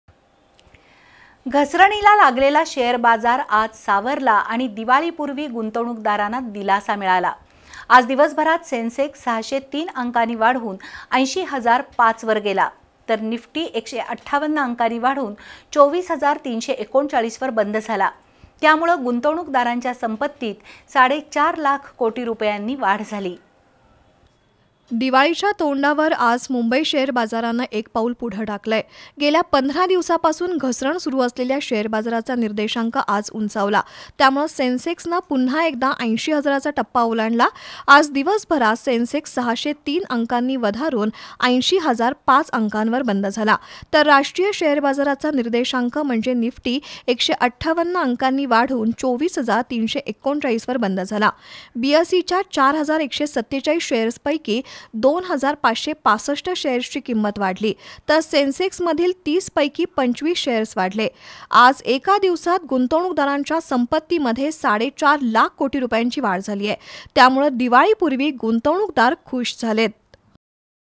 घसरणीला लागलेला शेअर बाजार सावरला
घसरणीला लागलेला शेअर बाजार सावरला, दिवसभरात सेन्सेक्स ६०३ अंकांनी वाढून, ८० हजार ५ वर, तर निफ्टी १५८ अंकांनी वाढून, २४ हजार ३३९ वर